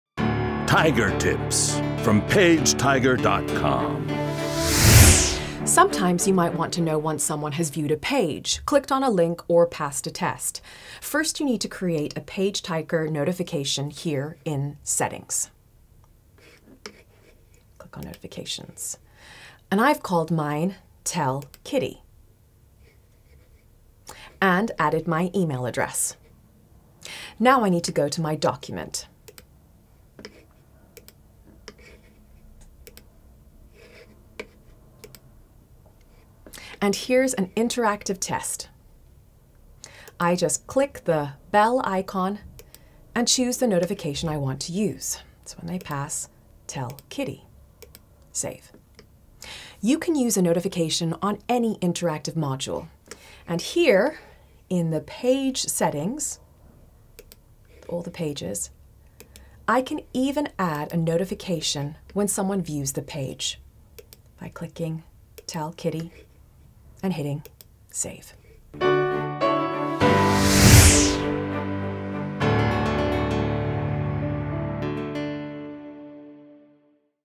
PageTiger - Triggering Notifications
Get a realtime email notification when a visitor views a page, clicks a link or passes a test.